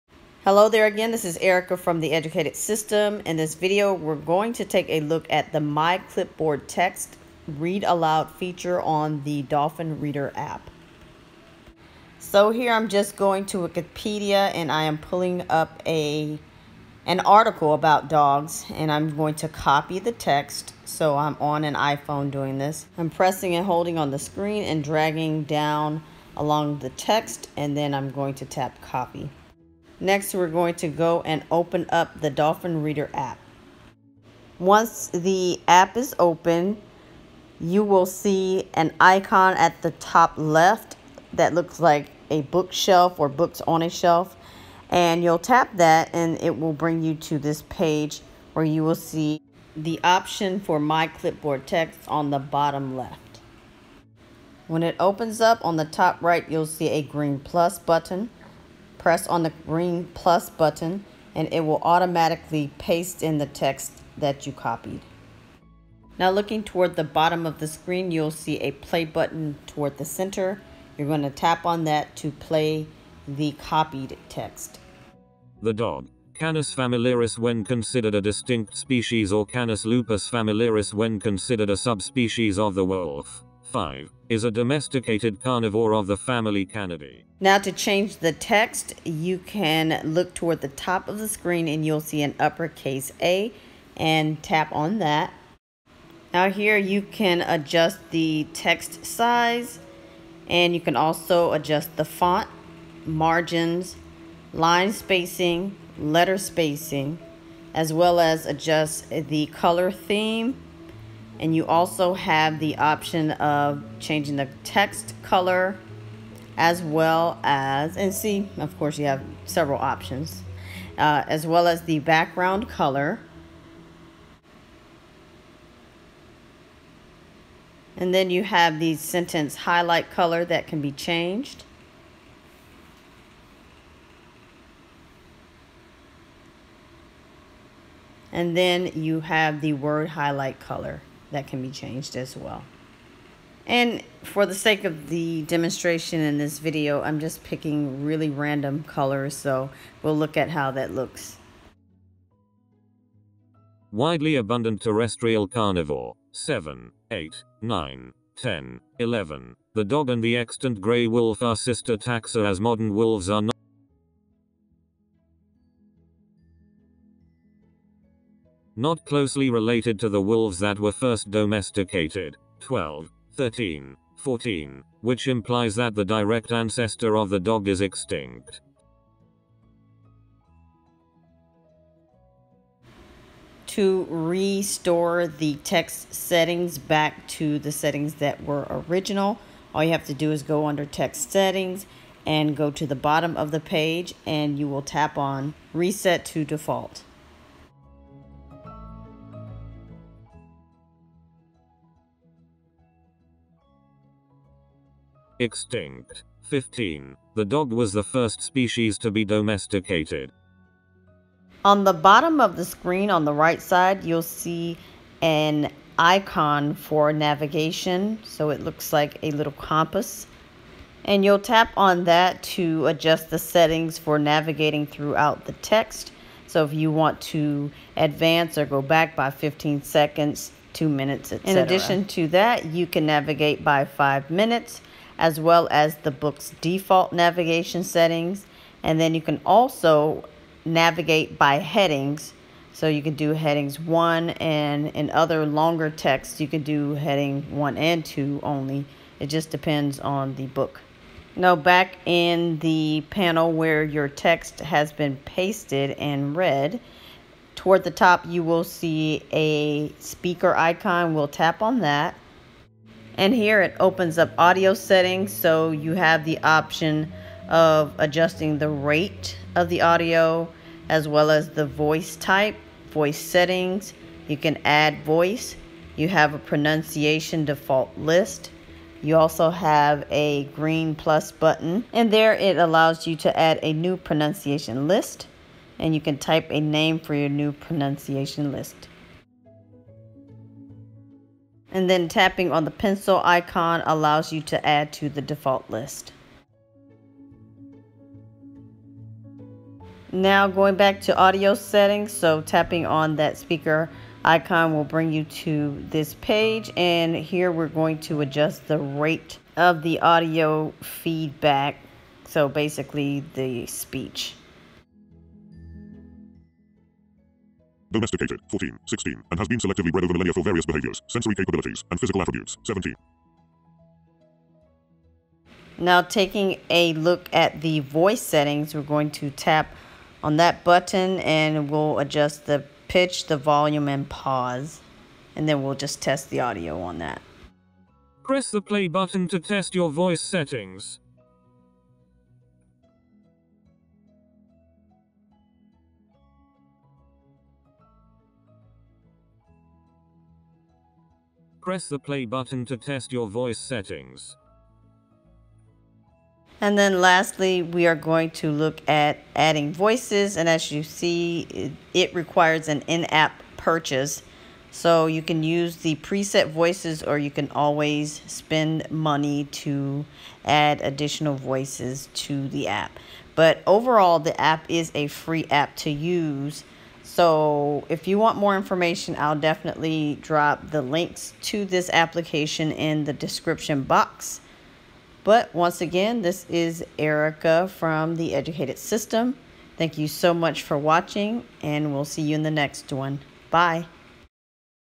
My Clipboard Texts: Dolphin Easy Reader App
In this video I demonstrate how to use the My Clipboard Texts feature in the DER app.